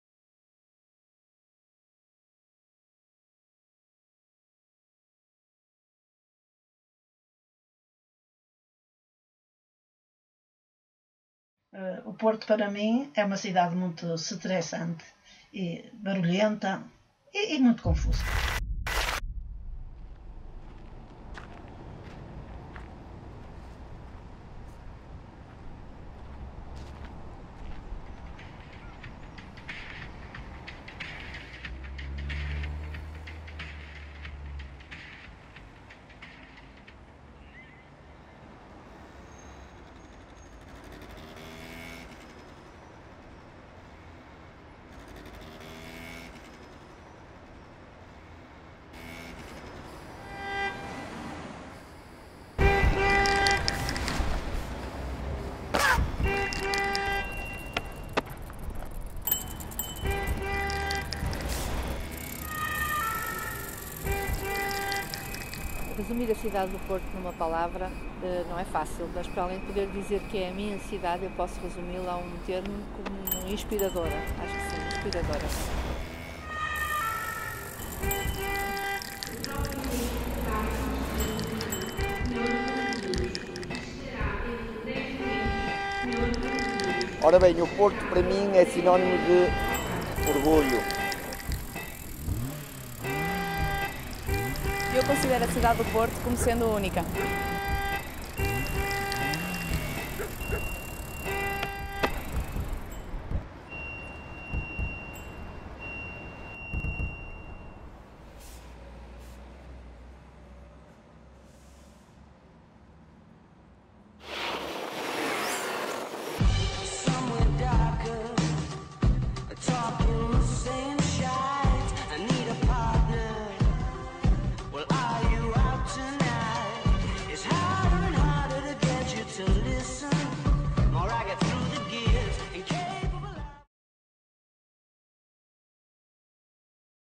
Short Film (Oporto)
A different look over the city of Porto. All has a perspective, people just need to find the good one.

Shot with Canon EOS 700D